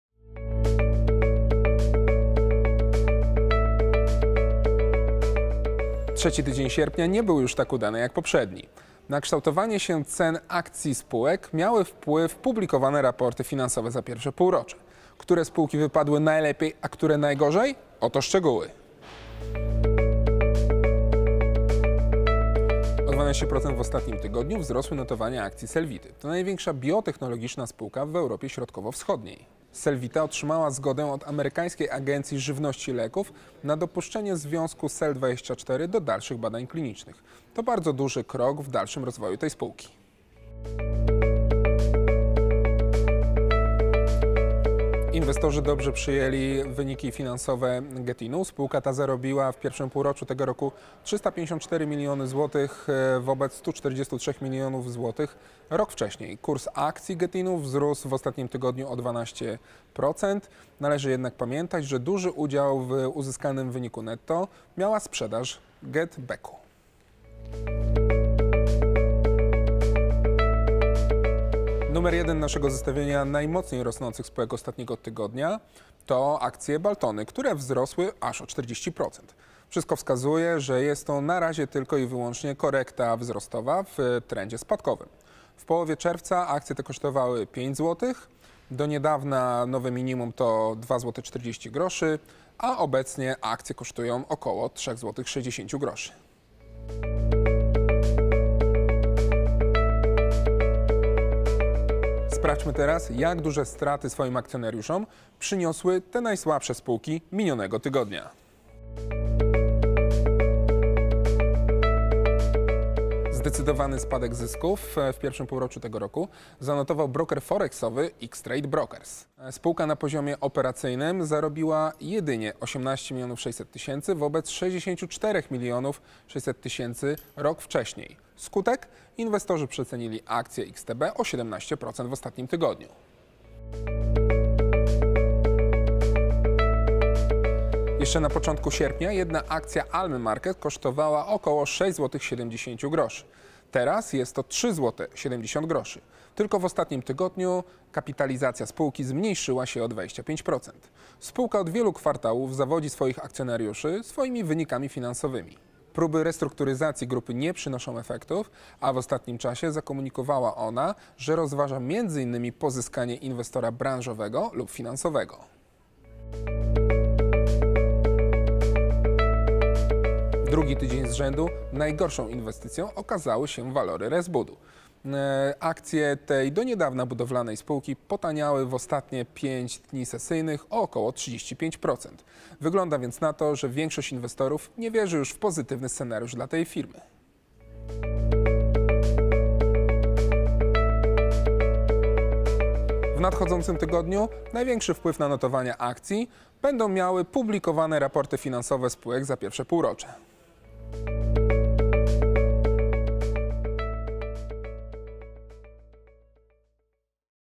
Spółki tygodnia, czyli giełdowy flash Rafała Irzyńskiego, #16 (19.08.2016)
Sprawdźcie, które spółki pozwoliły zarobić akcjonariuszom, a które przyniosły straty w trzecim tygodniu sierpnia.